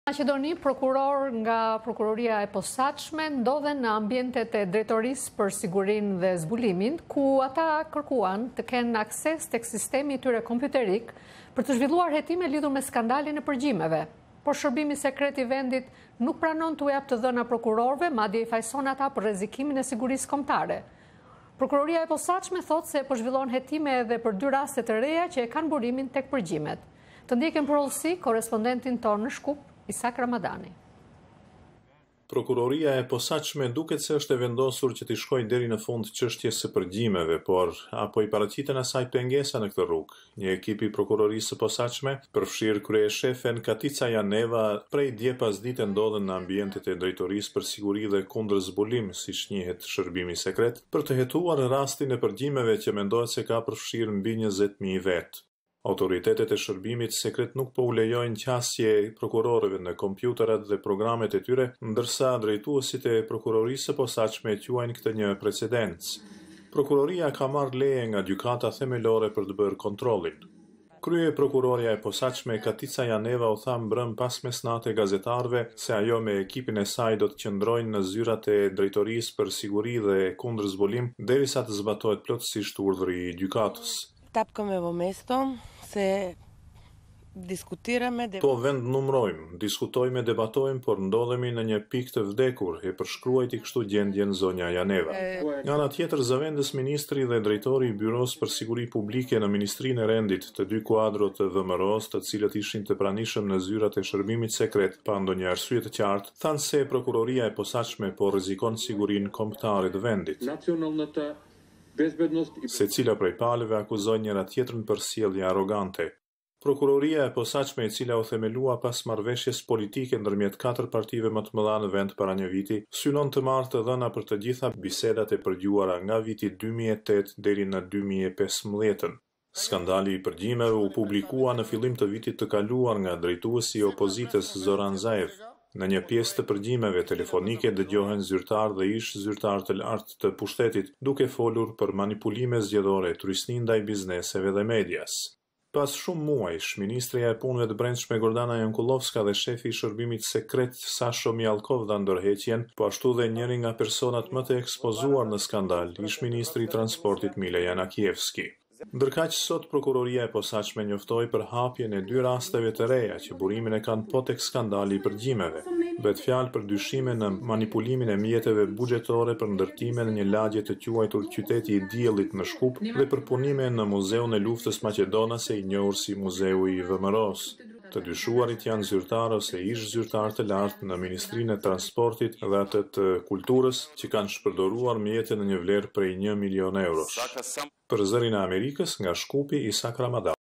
Maqedoni: Prokurorët kërkojnë qasje tek sistemi i kompjuterave të shërbimit sekret
Në Maqedoni, prokurorë të Prokurorisë së Posaçme prej dje ndodhen në ambientet e Drejtorisë për Siguri dhe Zbulim, ku po kërkojnë qasje tek sistemi i tyre kompjuterik për hetime të skandalit të përgjimeve.